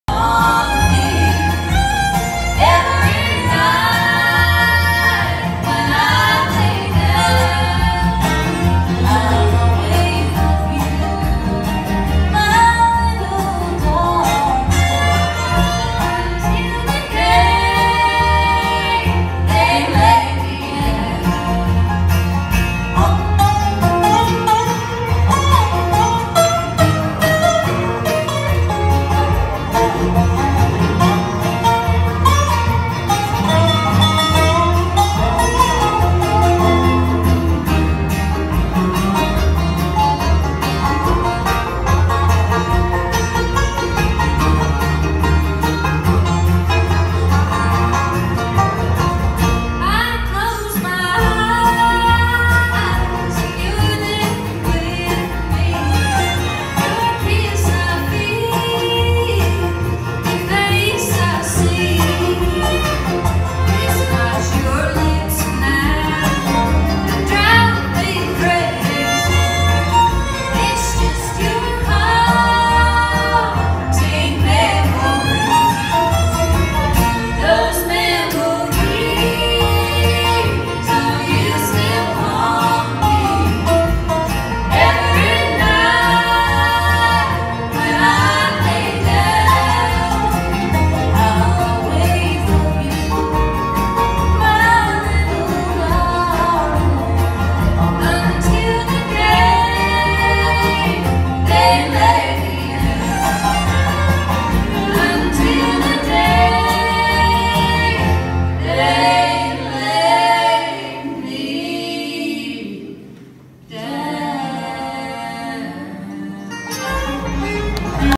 September 12, 2019
Rhonda Vincent and the Rage at The Cavern in Pelham Tn Aug 24th 2019